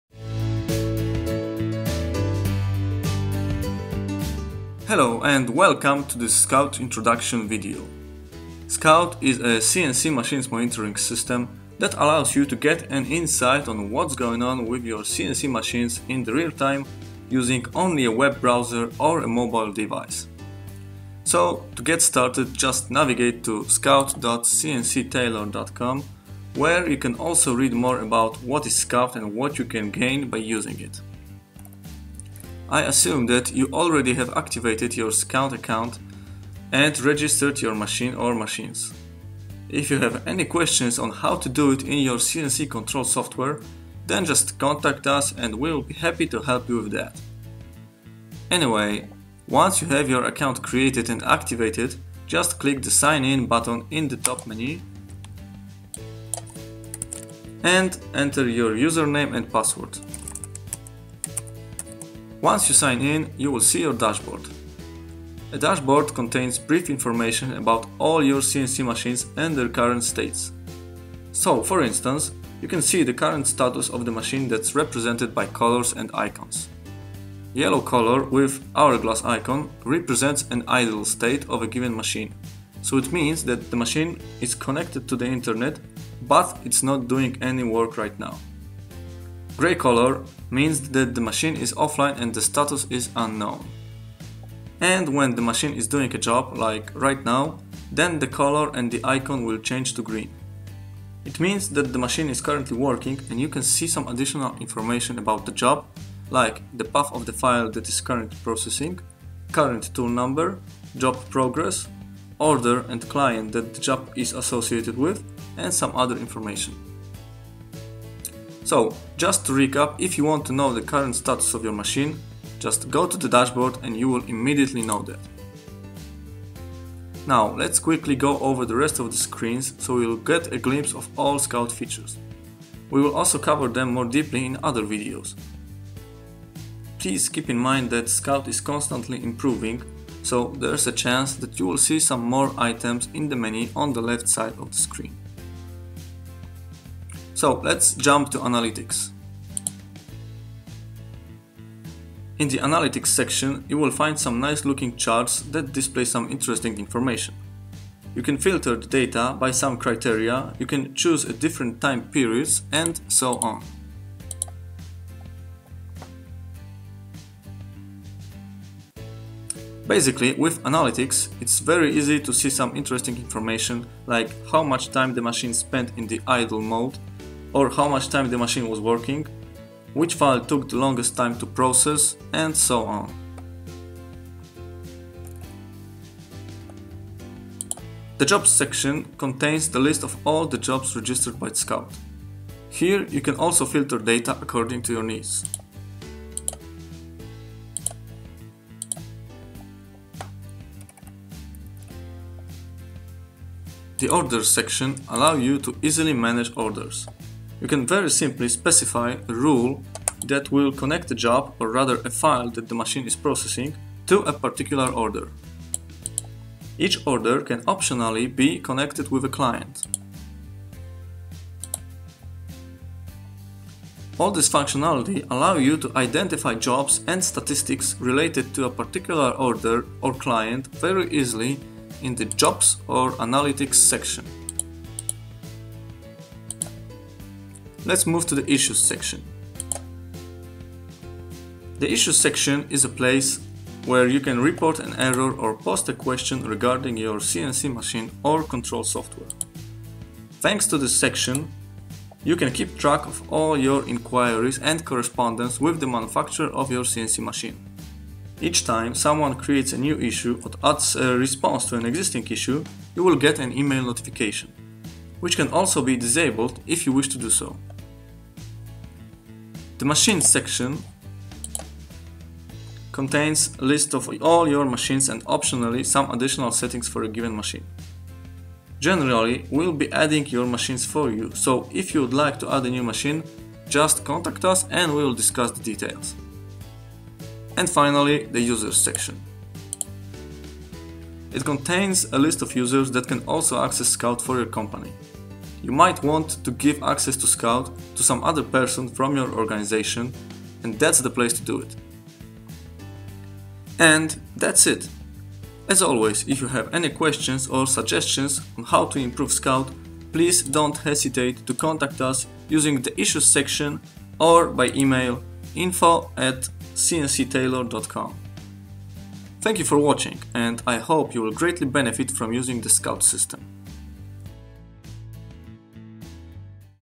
[EN] Scout - CNC machine monitoring - Introduction | Heidenhain | Fanuc | cncGraF
Introduction to Scout - real-time CNC machine monitoring system.

SCOUT IS A WEB APPLICATION (SAAS) FOR REAL-TIME CNC MACHINES MONITORING.

If you're using CNC machines in your business or you own a CNC shop floor, then Scout is for you. With Scout, you can easily check your machines' statuses and job statistics.

In this video, we will cover Scout basics.

Currently, Scout works with: Heidenhain, Fanuc, cncGraF, NC-Easy, InfoCAM and others.